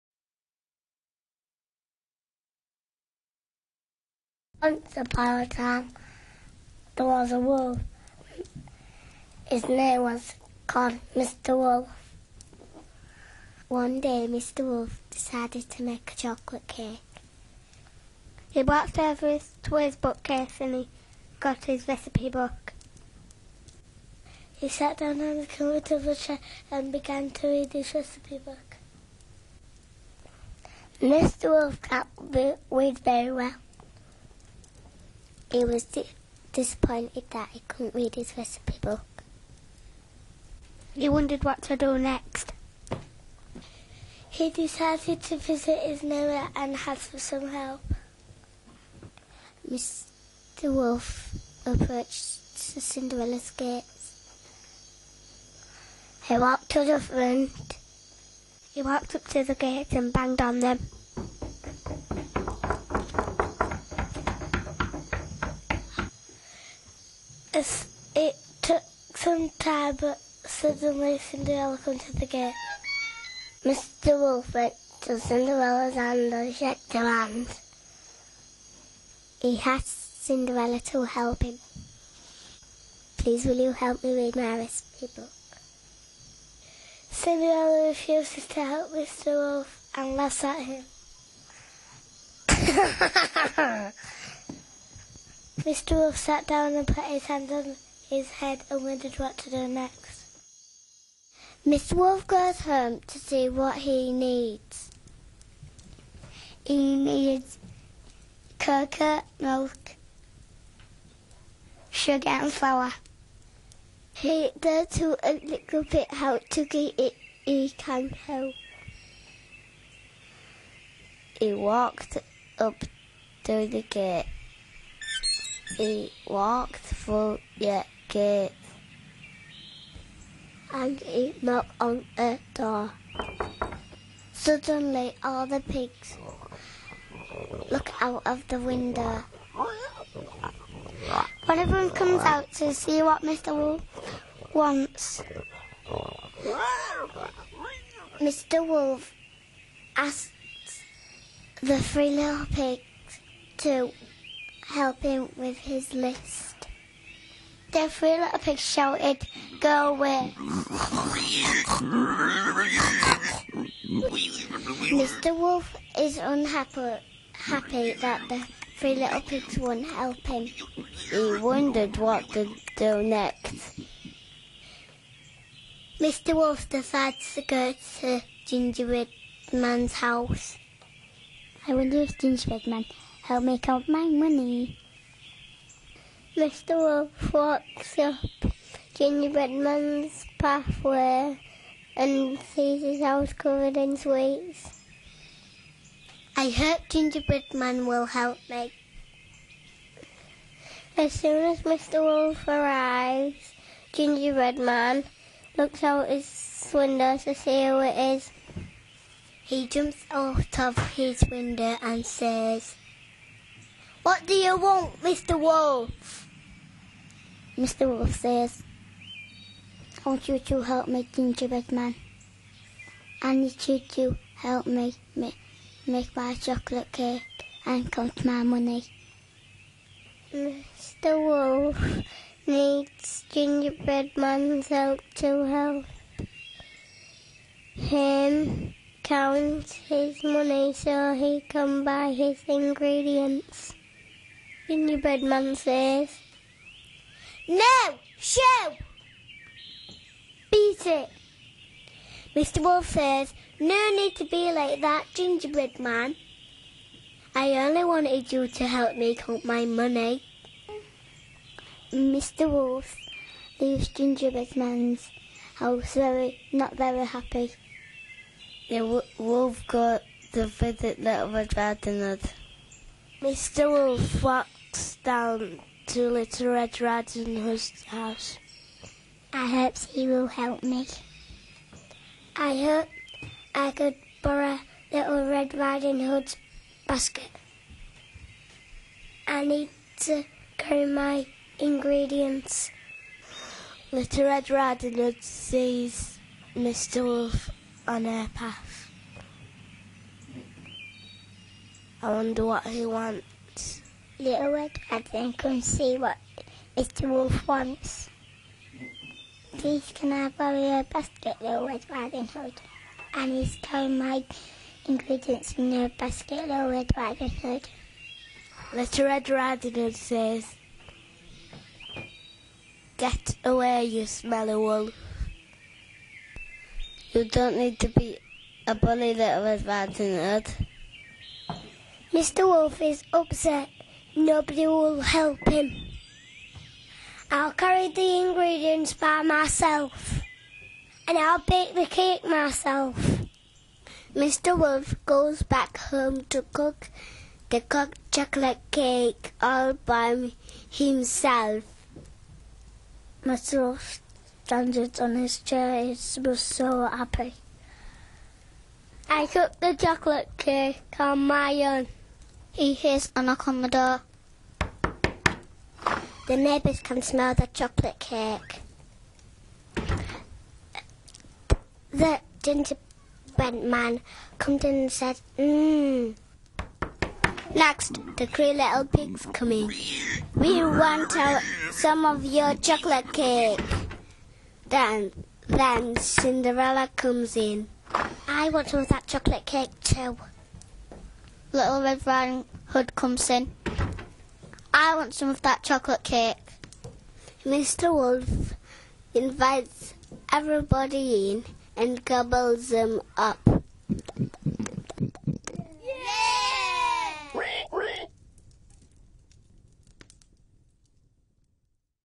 Mr Wolf's Chocolate Cake - Clifton Primary School - Hull
Made with year 1 pupils at Clifton Primary School as part of a speaking and listening project to improve numeracy and literacy skills through innovative use of new technology.

Project funded by Creative Partnerships - [June 2004]

Cafesociety.org
: Jon Robson